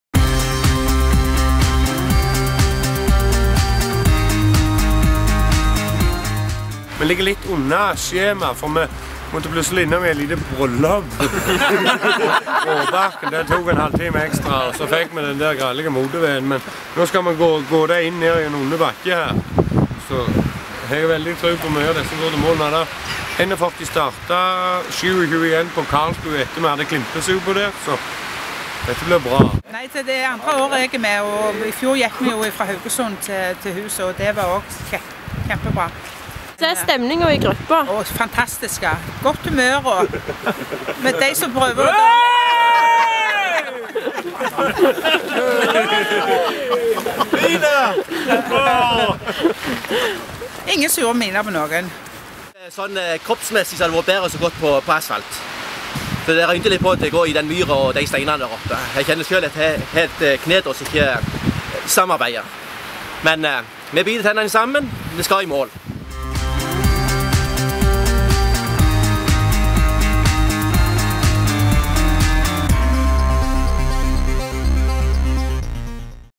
Mæland sprakk
Arvid Mæland og 40 andre starta i Egersund i natt for å gå den årlege turmarsjen på ti mil.